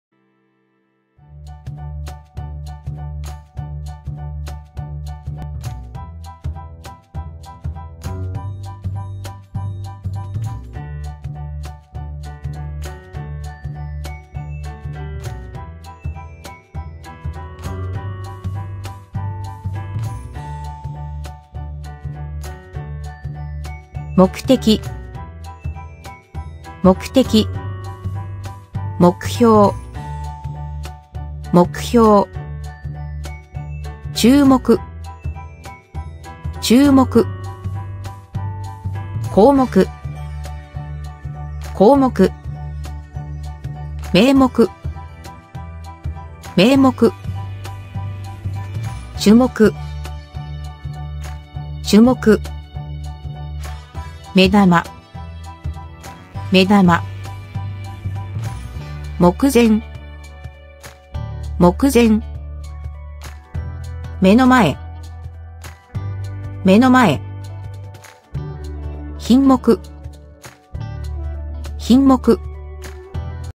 Learn and Write Kanji - 目 Eyes (52 of 80) 'Grade 1 Kanji'
This is a series of Kanji videos for those who want to learn the basics of learning and writing the Japanese Kanji. Learning and writing Kanji characters are as important as learning Hiragana and Katana. From the content you will find how to write Kanji and how these Kanji characters are placed side by side with another Kanji to form new words.

An important aspect of Kanji is each Kanji characters represent a particular word/concept or in some cases many words or concepts that when fixed with another Kanji will create a whole new meaning. This video provided some of those examples.

Another important aspect of writing Kanji characters is the stroke or the way Kanji are written. Like in the English alphabet, there are rules to follow in writing Kanji. So one does not simply start writing wherever one wants. The video already provided the basic guide for a better writing practice.

Lastly, do not worry on memorizing all the Kanji readings i.e. the onyomi and kunyomi readings . What is important is first knowing how to speak a word, then reading Kanji will become easier.

This particular series is focused on the Kanji taught in grade 1 Japanese schools in Japan. They are part of the jōyō kanji or the Kanji used on a regular basis and are guide to Kanji characters and their readings. As the grade level progress, Kanji characters become more complicated and thus making them more challenging.

目
kun: め, -め, ま-
on: モク, ボク
meaning: eye, class, look, insight, experience, care, favor